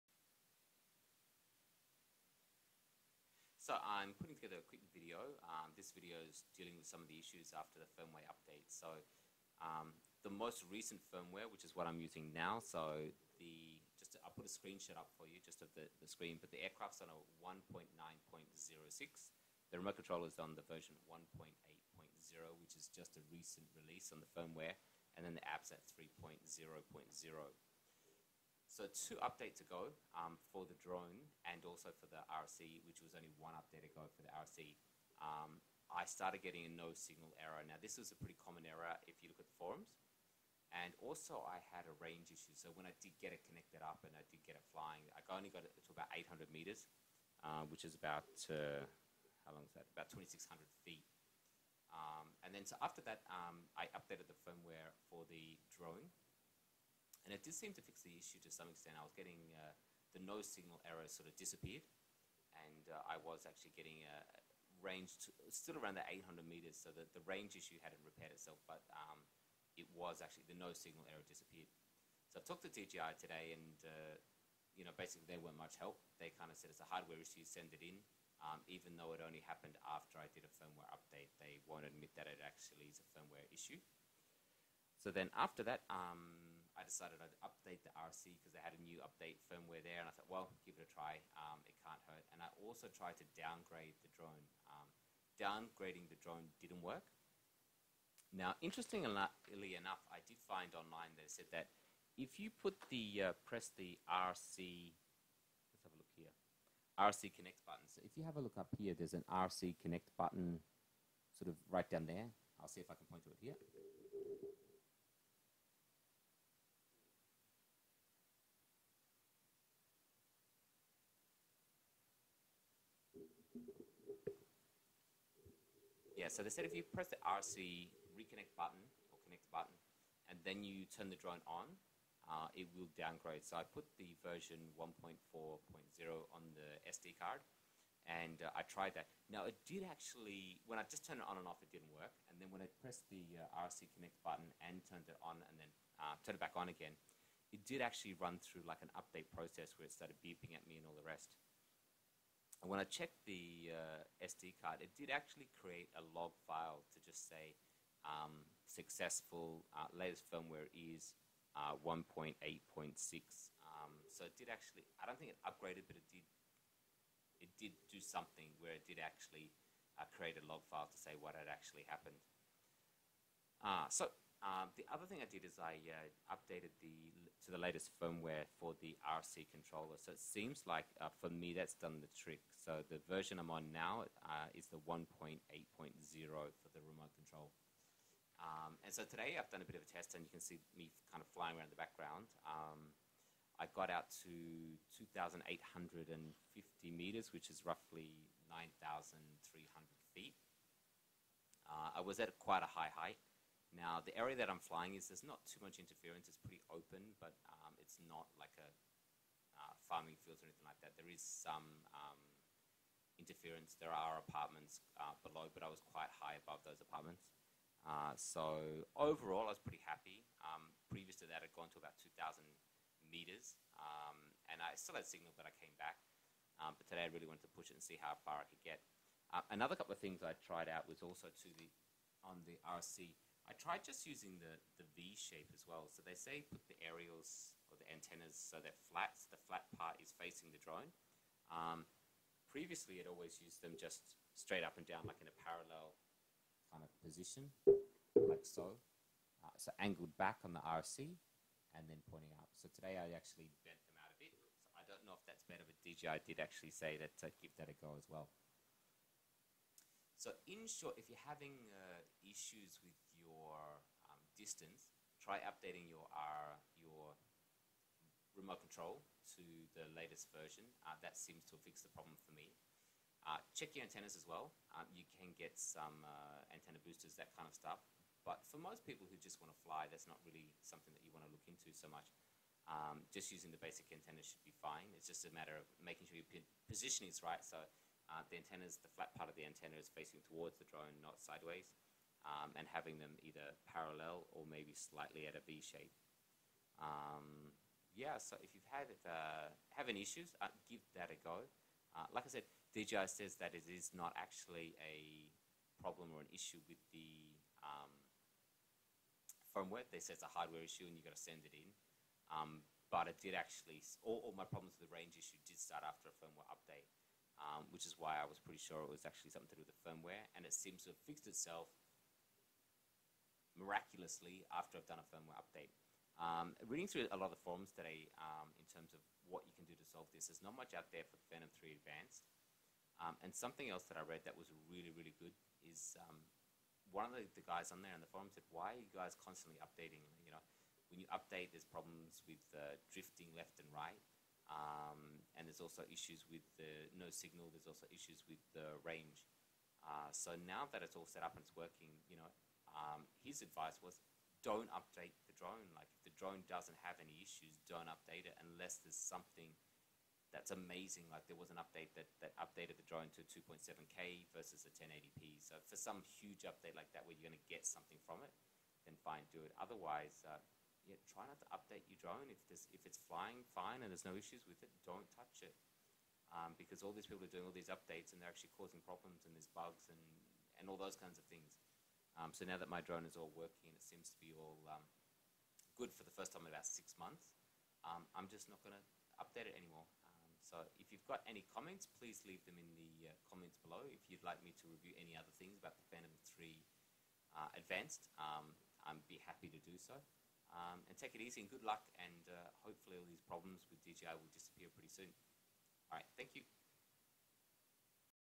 Phantom 3 Range issue fix
Recently some Phantom 3 pilots have been having issues after firmware updates. In this video I talk about some of the issues I was having and the steps I took to get my drone up an running normally again.